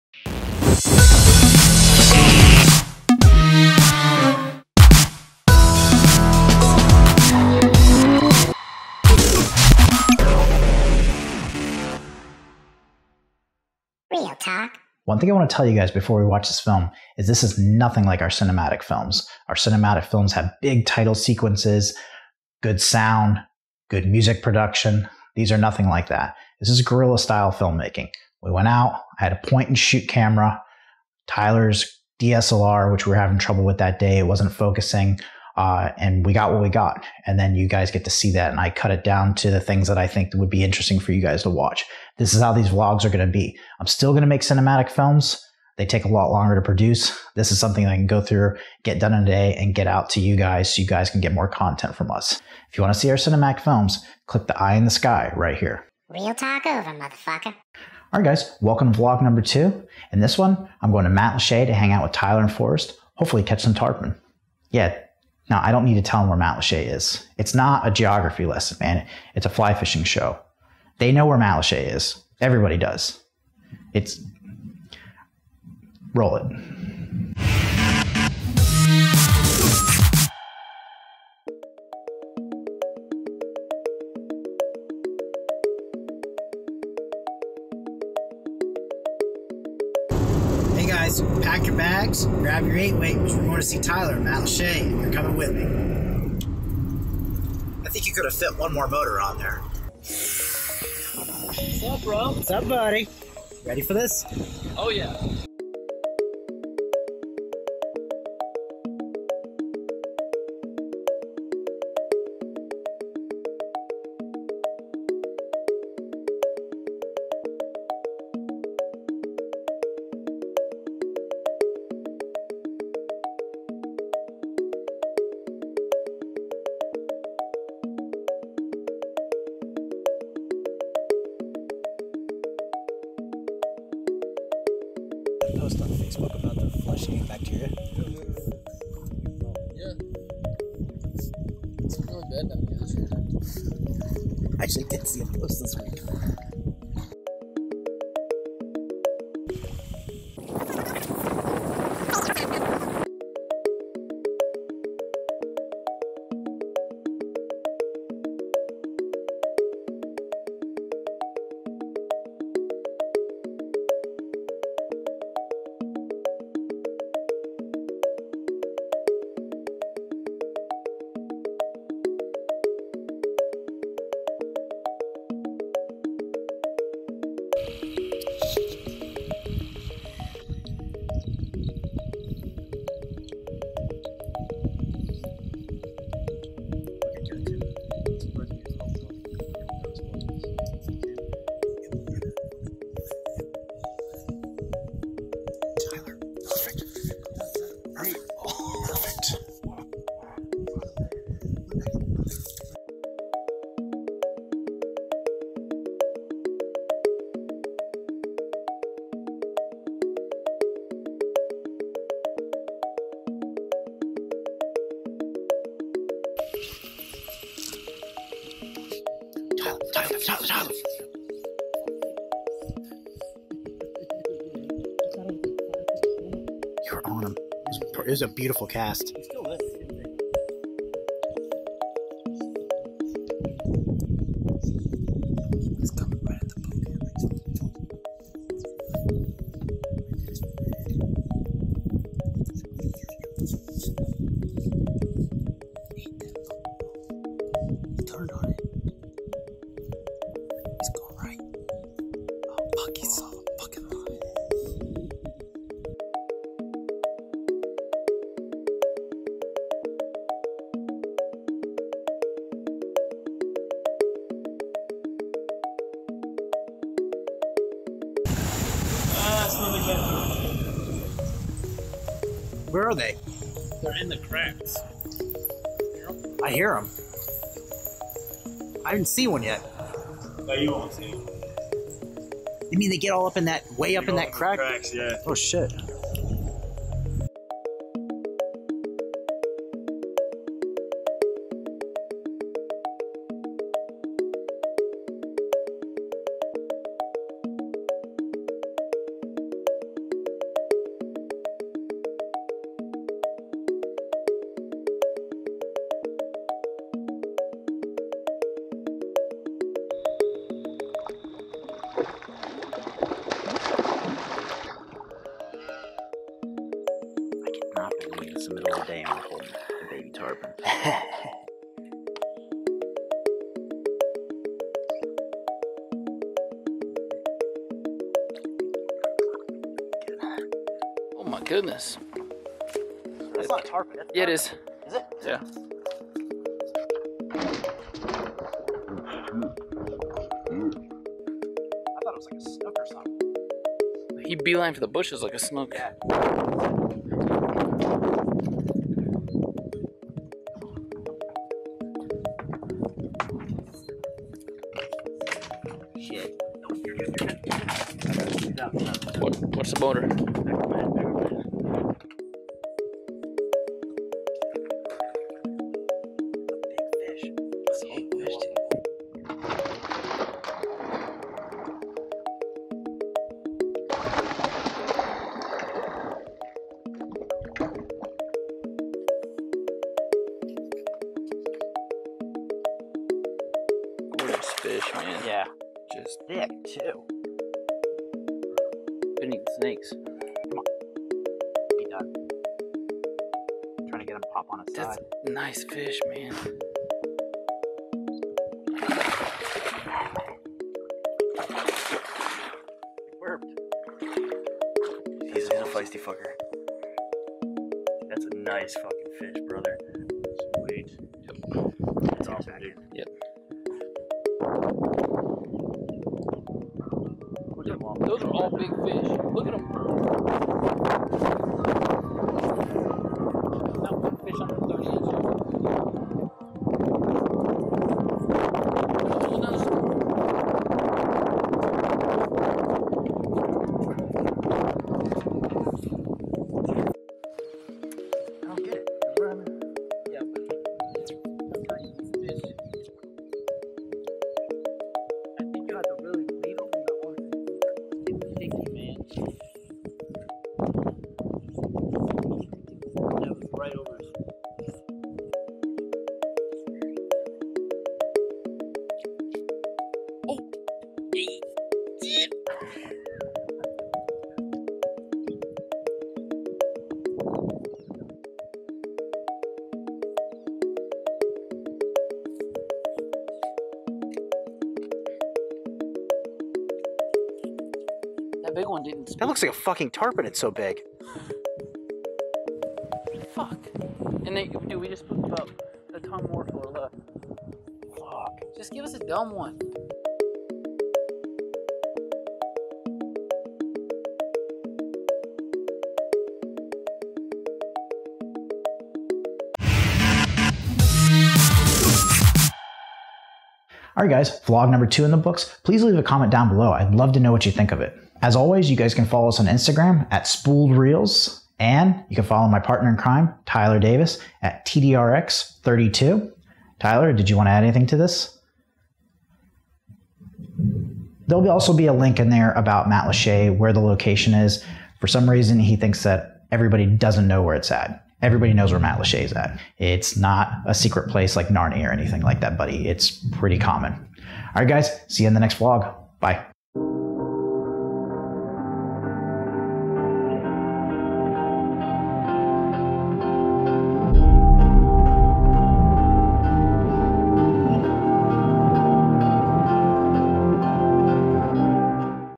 Insane Baby Tarpon on Fly - Spooled Reels Vlog - SRV #2
SRV #2 - I go to Matlacha, Florida to fly fish for baby Tarpon with Tyler and Forrest.

Google Map of Matlacha even though you already know where it is